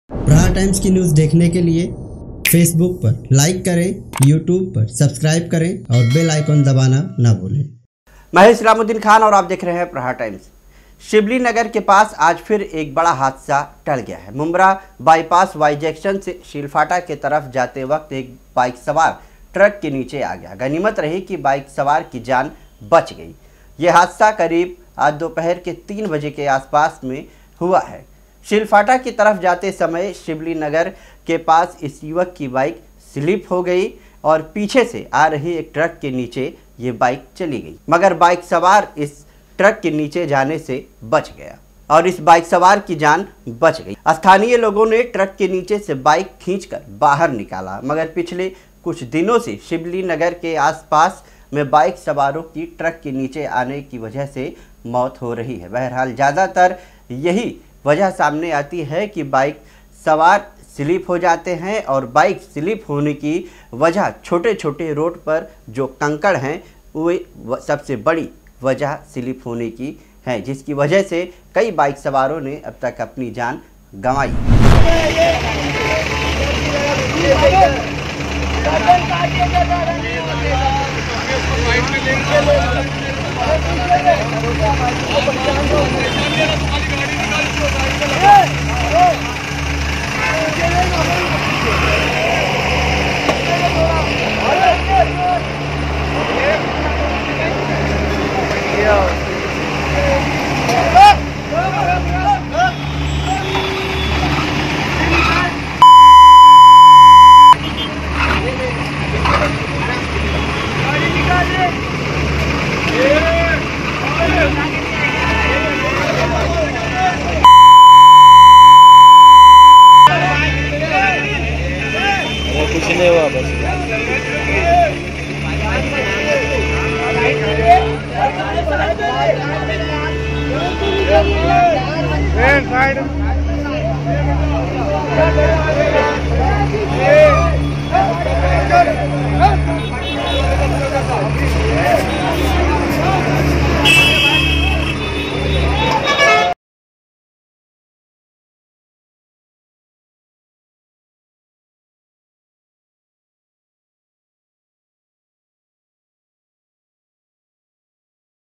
शिबली नगर के पास टला बड़ा हादसा बची युवक की जान || Islamuddin Khan|| Aapka Prahar Times
शिबली नगर के पास टला बड़ा हादसा बची युवक की जान ||Islamuddin Khan

शिबली नगर के पास ट्रक के नीचे फिसल कर एक बाइक सवार आगया गनीमत रही कि बाइक सवार की जान बच गई